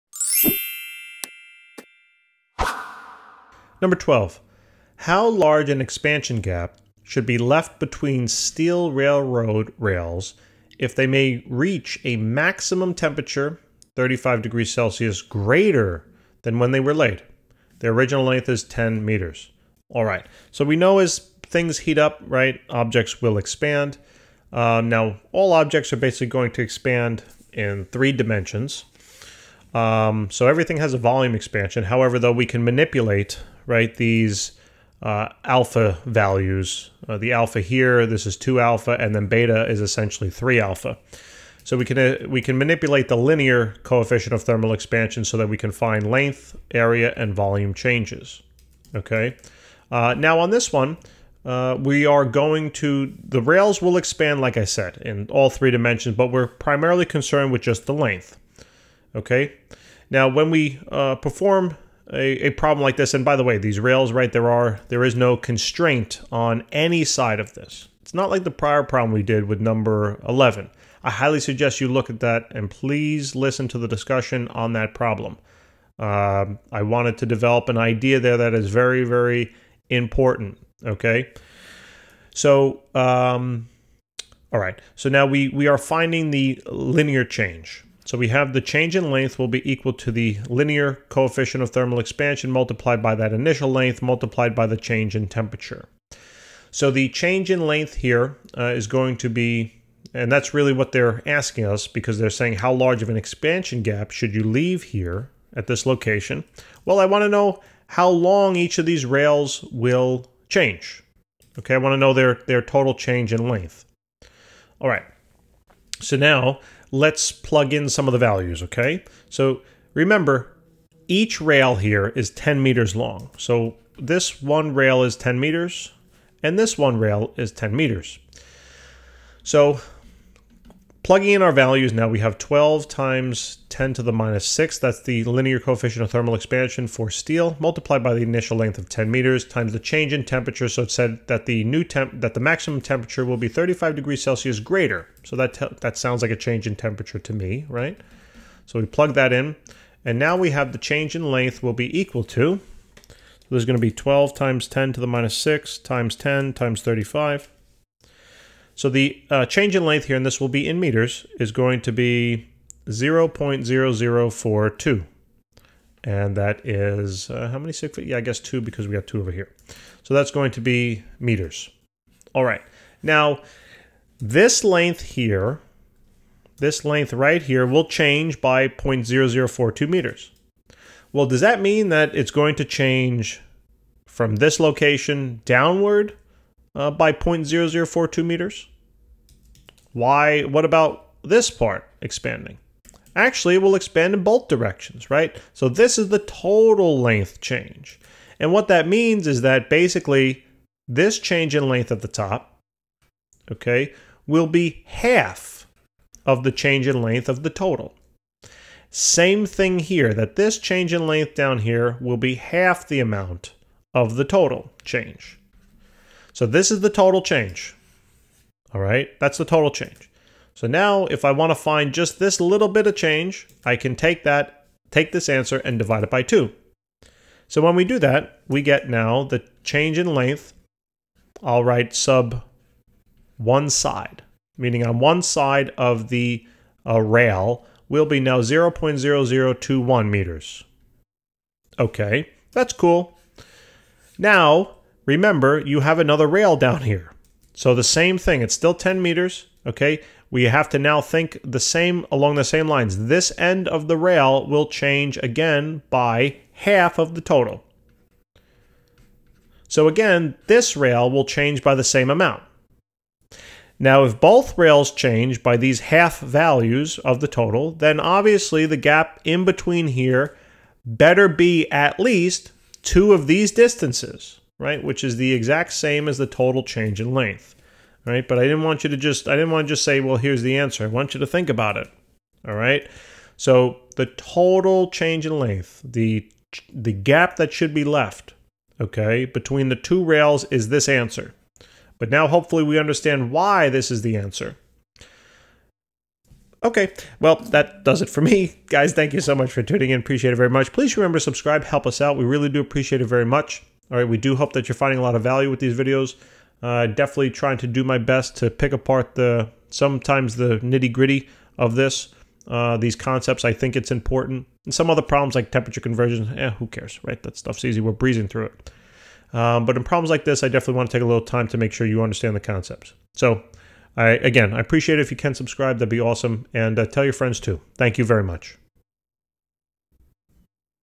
13.12 | How large an expansion gap should be left between steel railroad rails if they may reach a
How large an expansion gap should be left between steel railroad rails if they may reach a maximum temperature 35.0ºC greater than when they were laid? Their original length is 10.0 m.